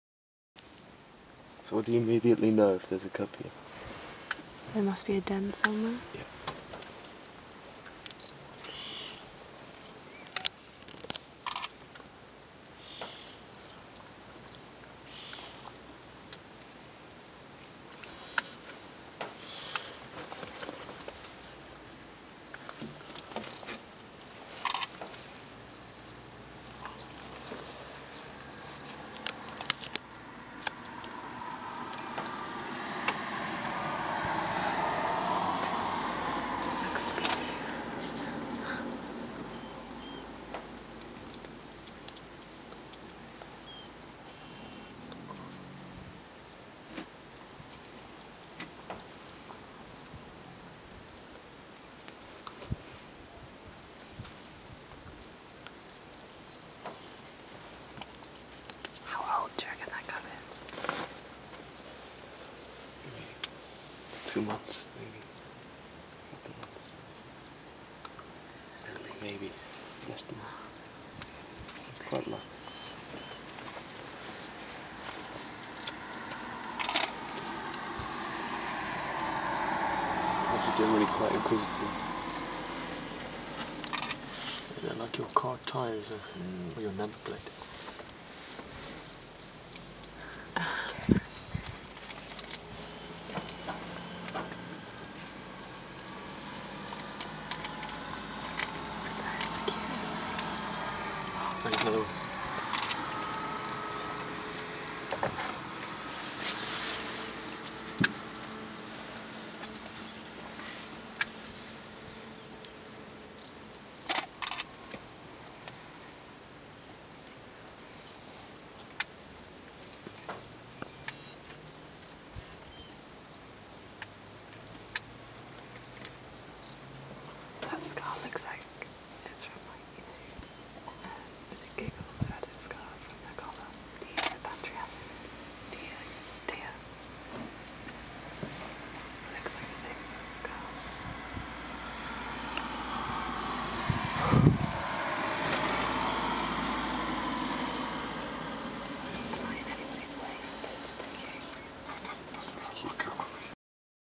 Hyaena and baby in Kruger
A hyaena and her baby in Kruger National Park in South Africa.  Had this incredible sighting whilst training for my FGASA